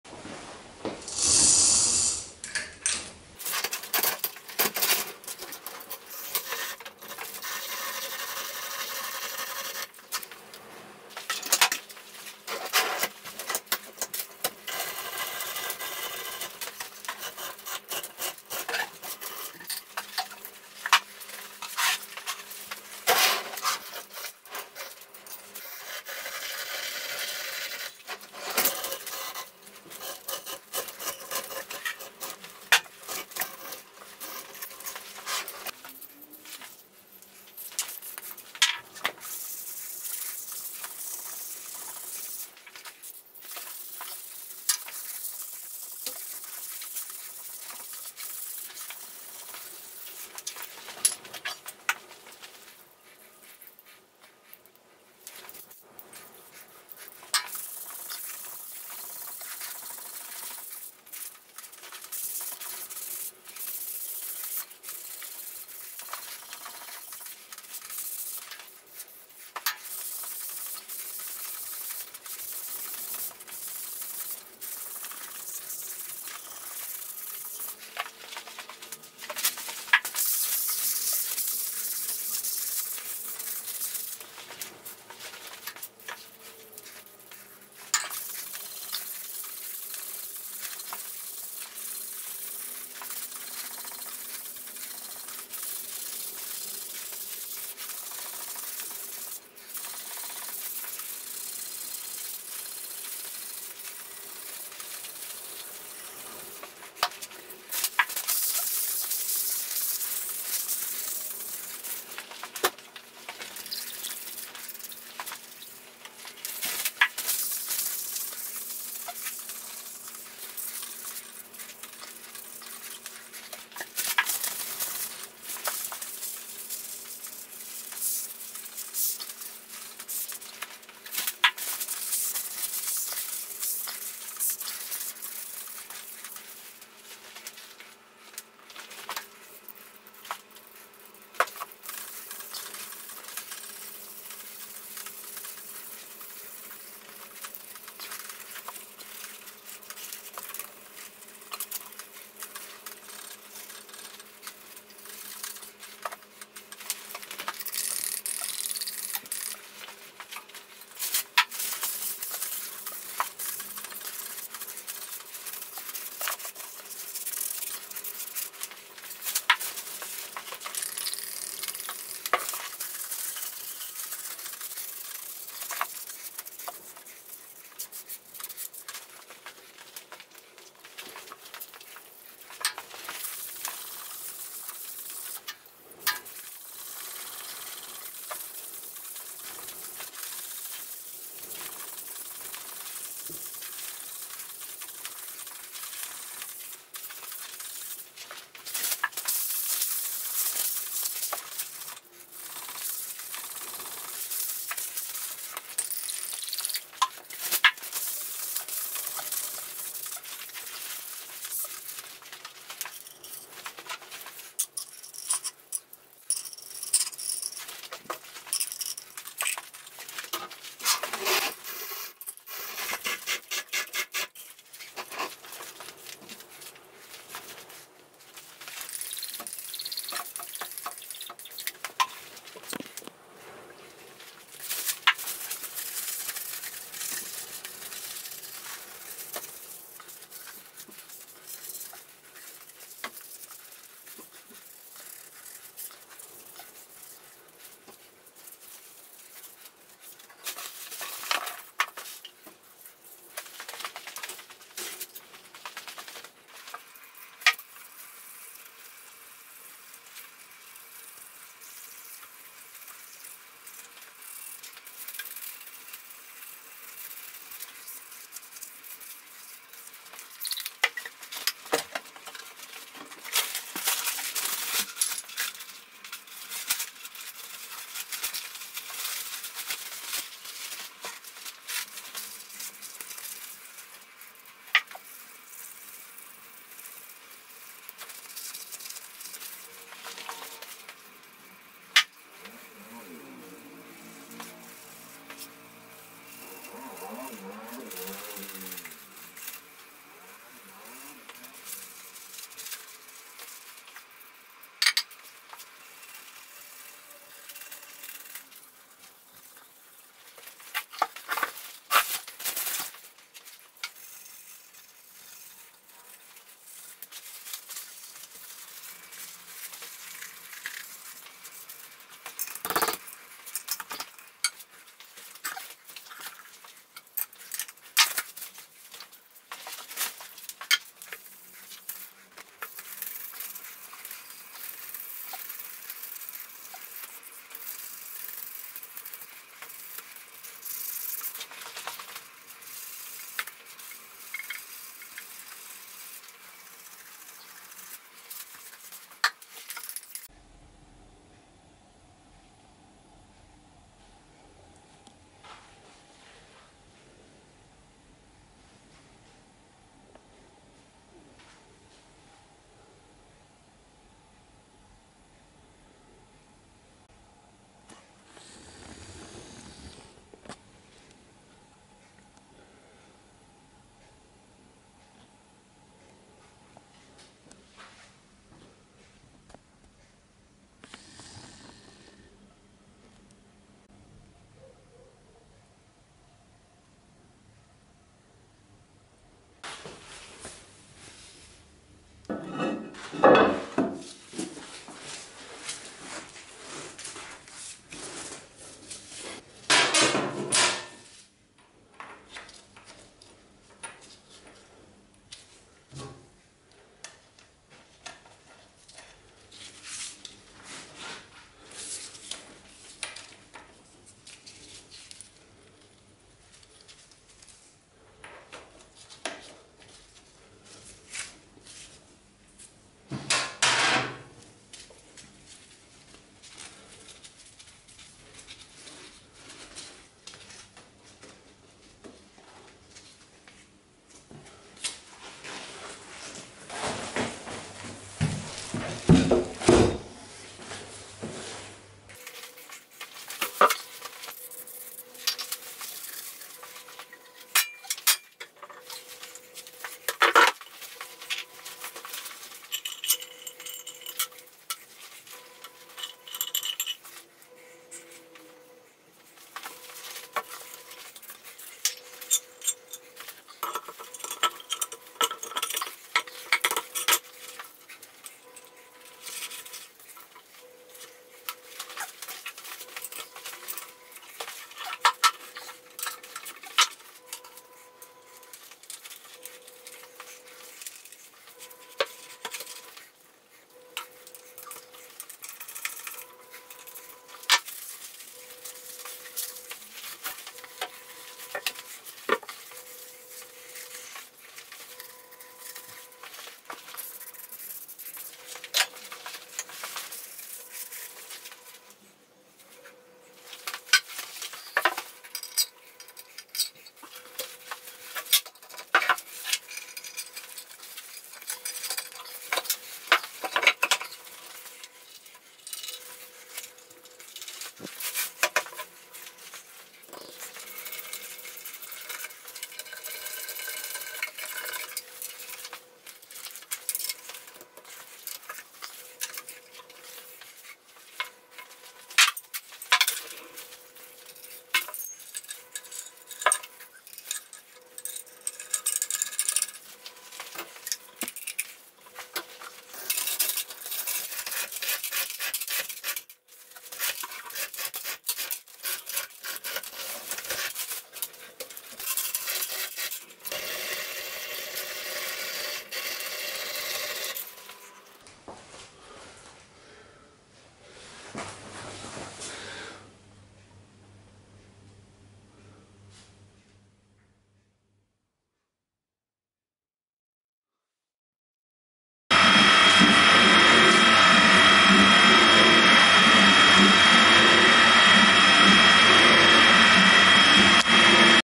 Restoration of INTOS FNGJ20 accesories PART 1
First video in a series of several.

A quick 'restoration' of my newly acquired accessories for my Intos fngj20 tool room milling machine.